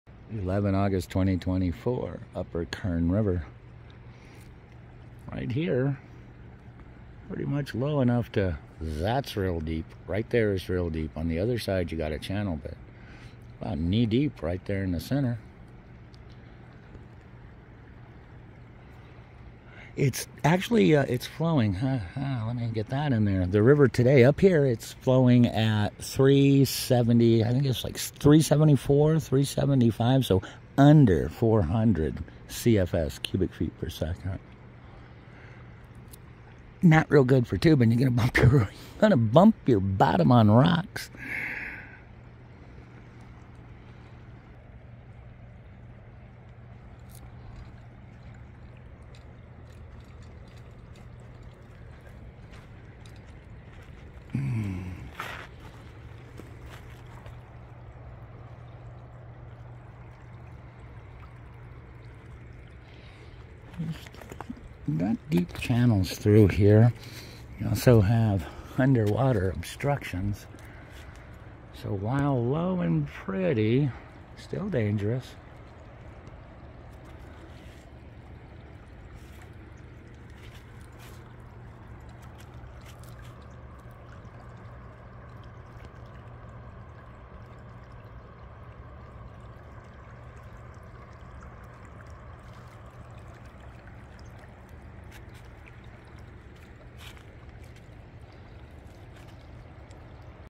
The Kern River
The river is low today, at around 375 cfs. The weather is good and the 178 is open in both directions.